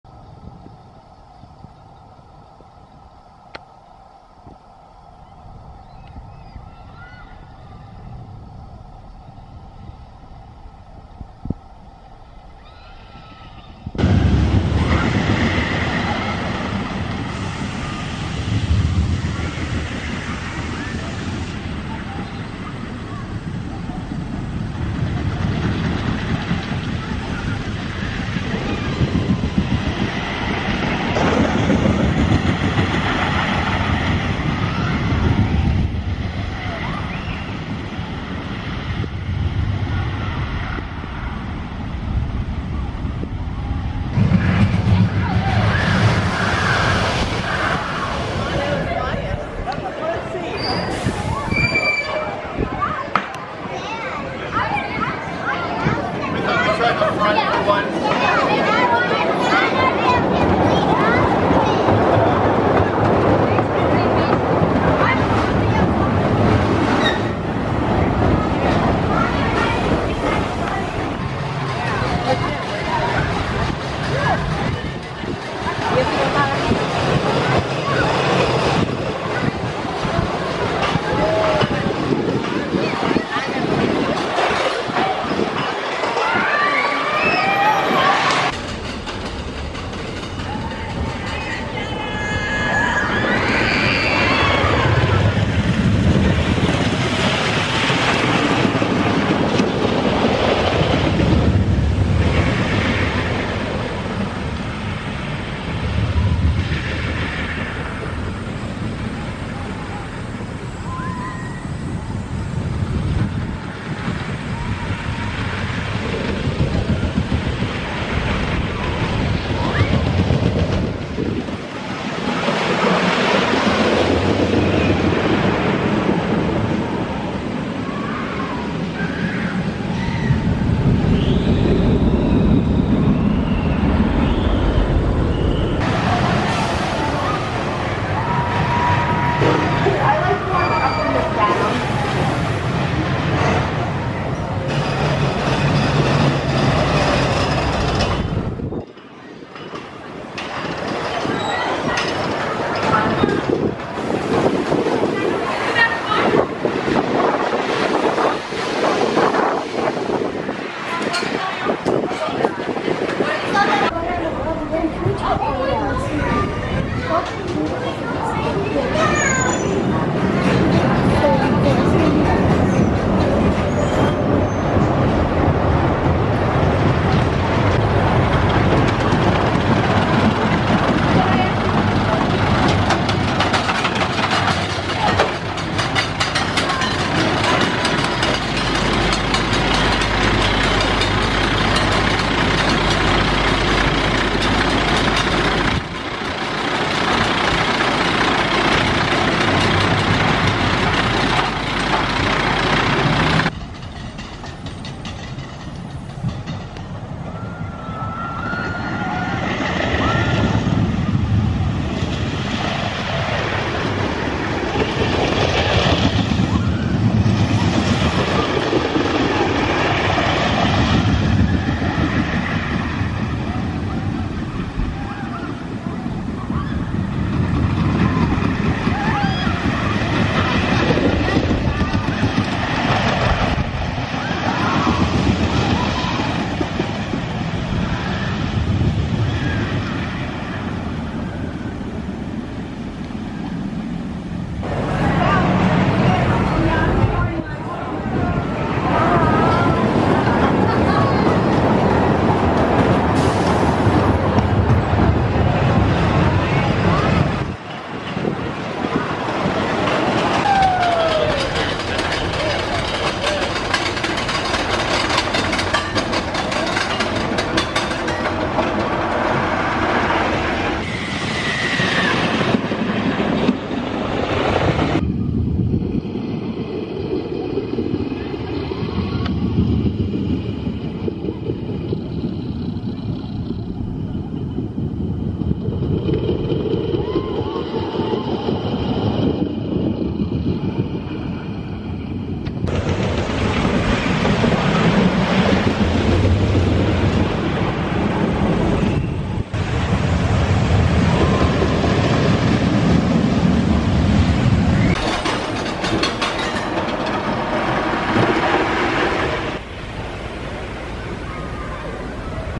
Jack Rabbit Off-Ride Footage, Seabreeze Classic Wood Coaster | Non-Copyright
Jack Rabbit at Seabreeze is one of the oldest wood coasters in the world and one of the first to feature upstop wheels.  Designed by John Miller, this coaster runs along the parking lot and into the woods.